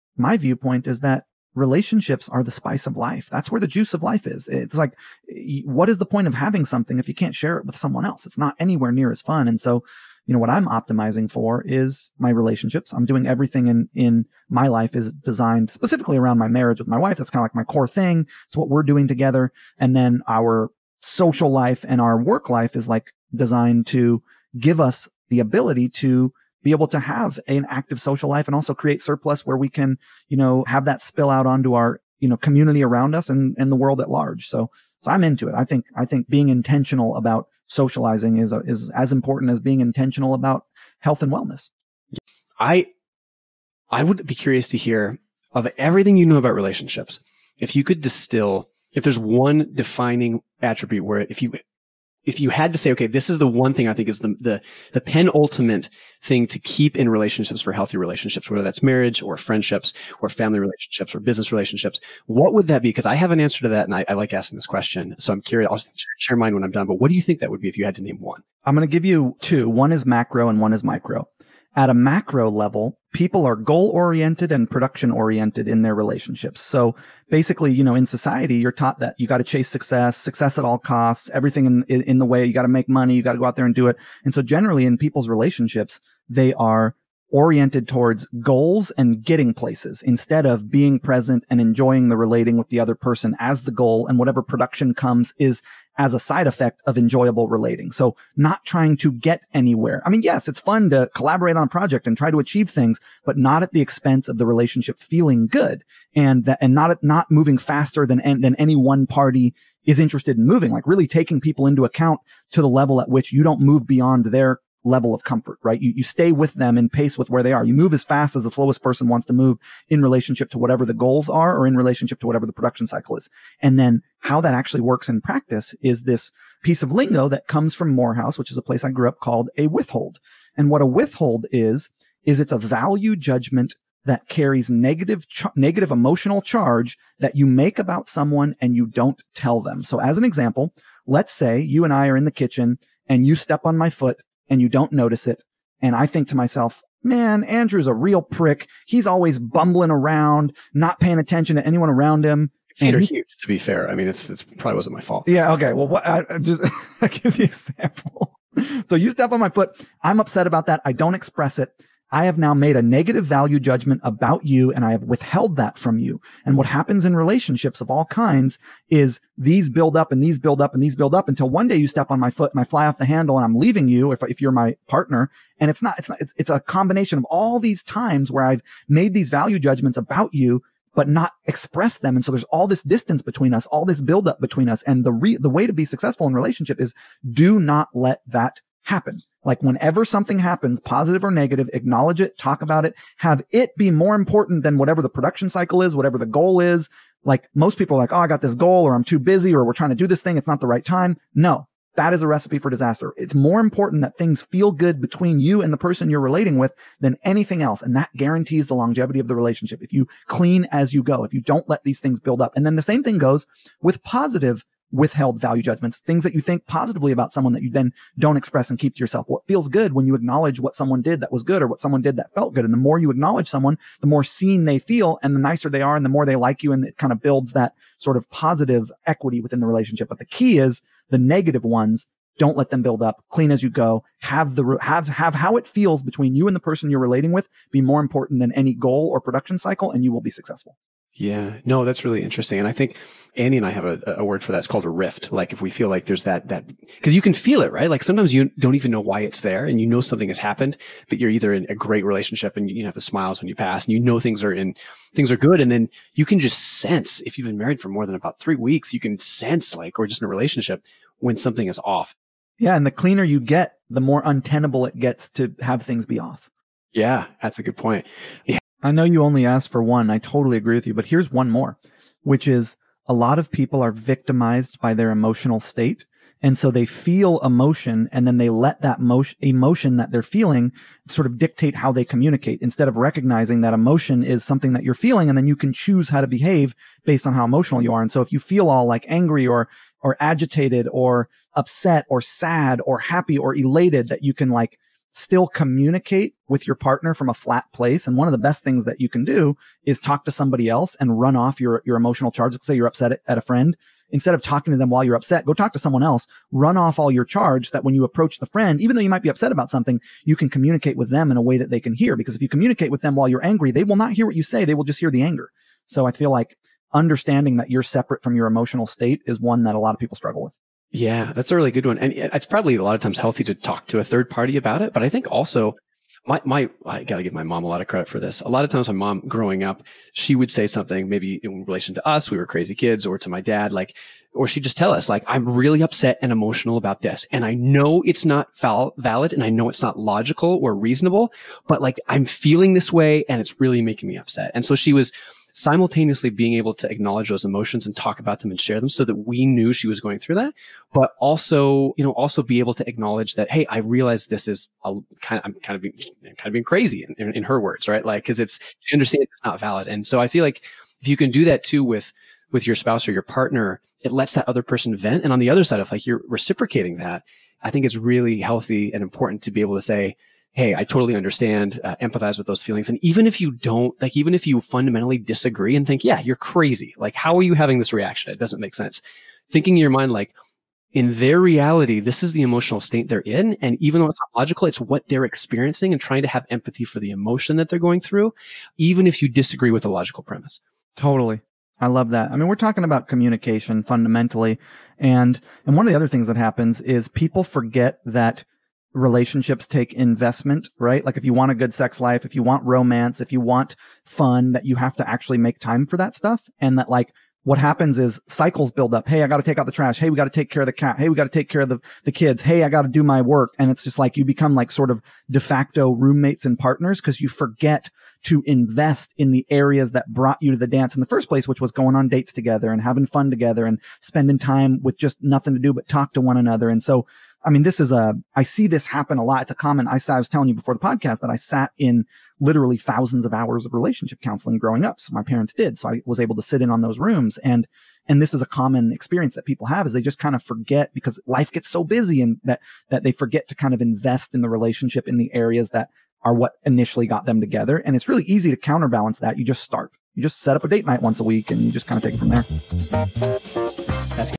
The Key To Winning Relationships In Life and Business
New Ecommerce Fuel podcast with my good buddy Andrew Youderian!

We cover:

You’ll learn:

- What it was like growing up on a hippie commune
- How communal living works
- The number one important attribute in relationships
- How to cultivate solid relationships in your life